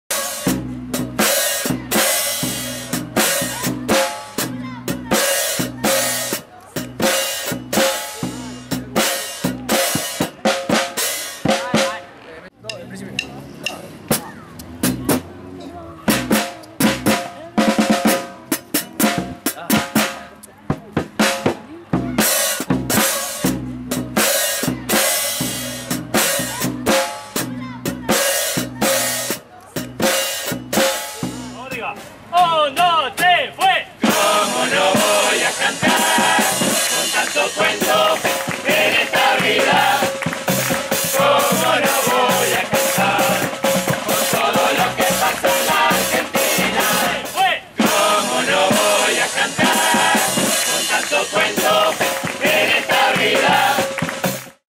Murgas
El domingo 16 de marzo, en la Plaza San Mauricio de San José, la organización Raiz Popular junto a vecinxs de San José, Belgrano y Pedro Molina dieron comienzo a la Feria Barrial Comercial, alli estuvo presente la Murga La Caciqueña.
Murga del Barrio Pedro Molina, siempre presente en las actividades del barrio.
Aplausos para la murga...